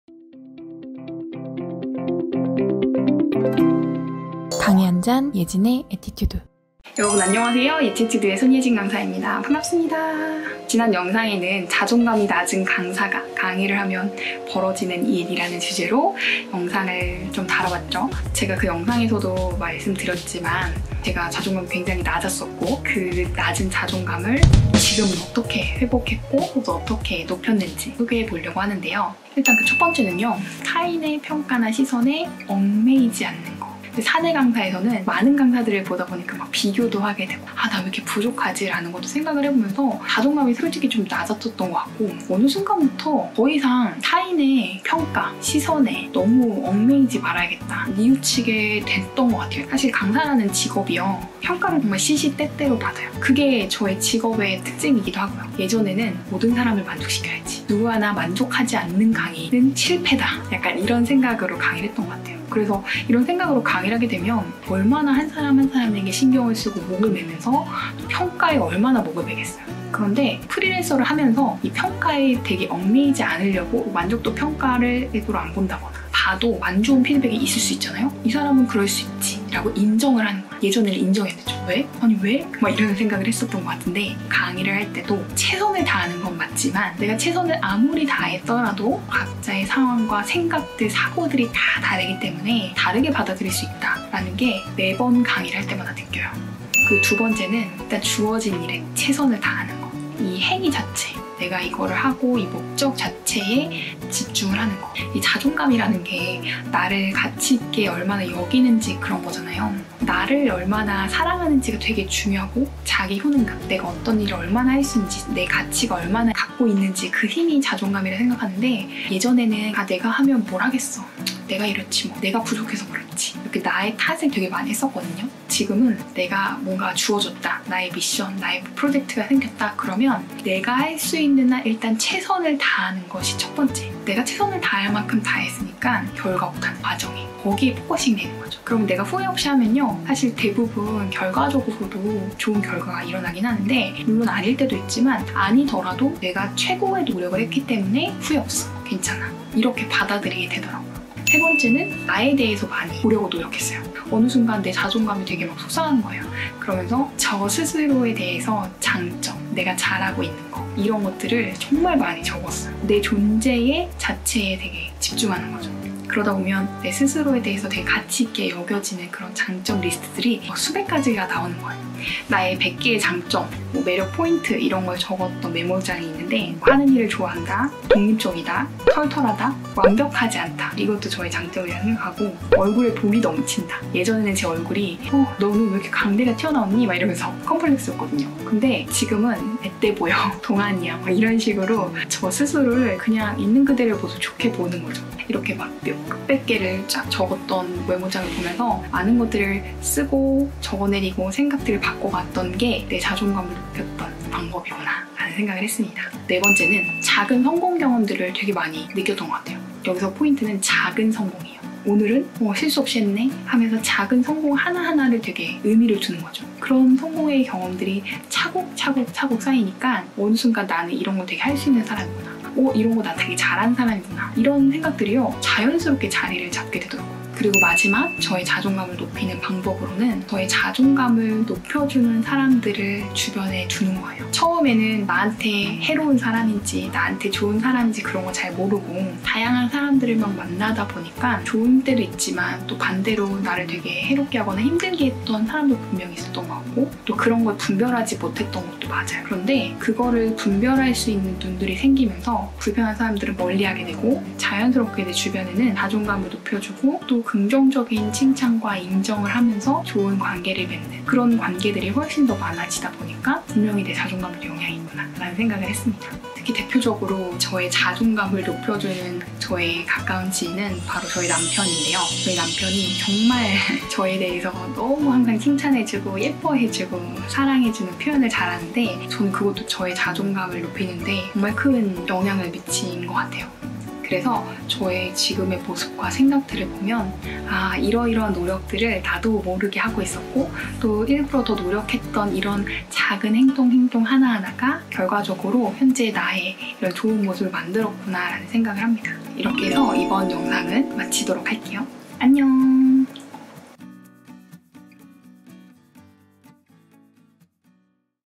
자존감 높이는 5가지 방법 / 강의한잔
안녕하세요 예티튜드 손예진 강사입니다 :)
오늘은 지난 영상(자존감 낮은 강사가 강의하면 벌어지는 일 )에 이어서
손예진강사의 자존감 높은 강사가 되기 위한
5가지 방법을 소개해드립니다~

제가 삶을 살아오면서 이 5가지 방법이
저의 낮은자존감을 높이는데 가장 중요한 역할을
했더라고요! 사실 의식하지 않고 한 행동이라기보다는
삶을 돌이켜보니 다양한 방법 중에 이 방법들이
저의 현재의 모습을 만든 것 같아요.

앞으로도 멋진 손예진강사로! 성장하겠습니다.
늘 응원해주시는 구독자님들게 감사합니다 :)

강사, 강의 관련 궁금한 점 있으시면 언제든지 댓글 달아주세요~
여러분과 자주 소통하고 싶은 손강사 입니다^^